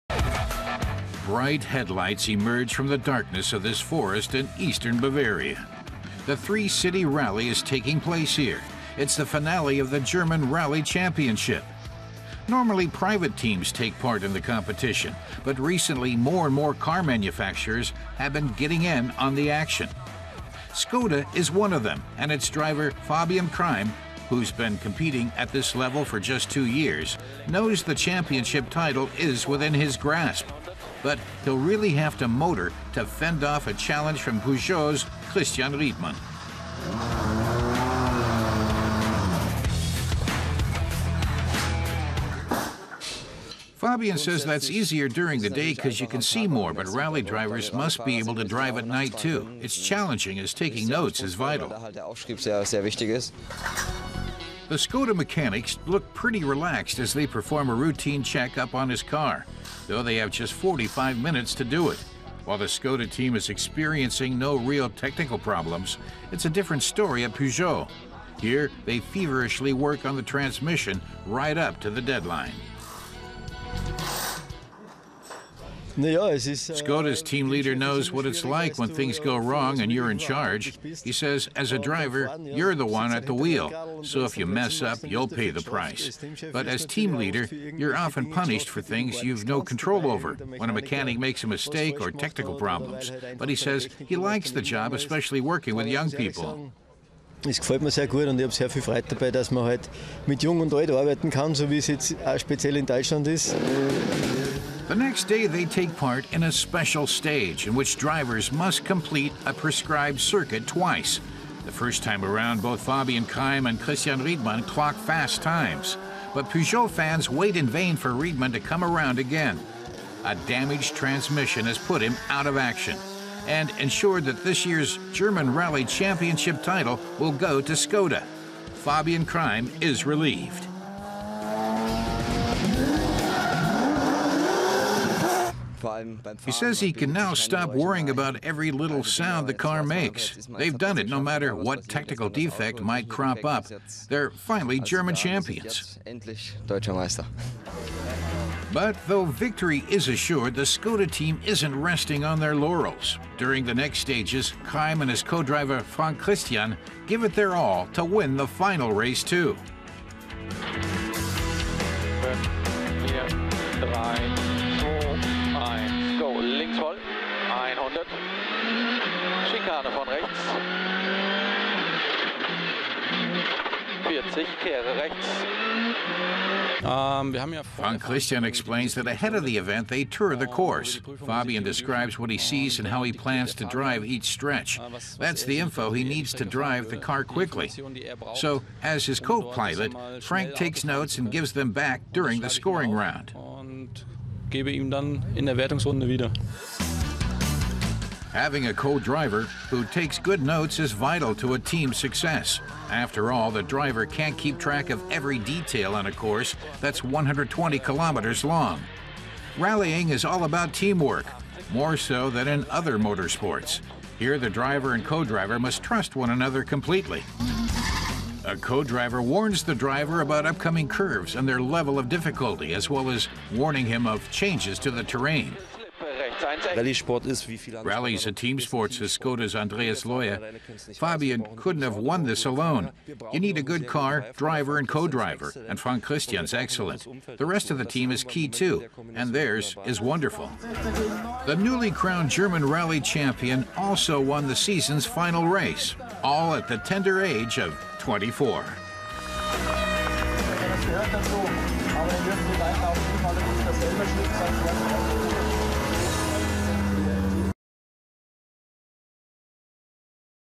Fast: German Rally Championship Finals | Drive it!
The German Rally Championships used to be a series in which mainly private teams competed. But more and more car manufacturers are getting in on the action. We follow one of Skoda's young drivers, Fabian Kreim, as he vies for the title.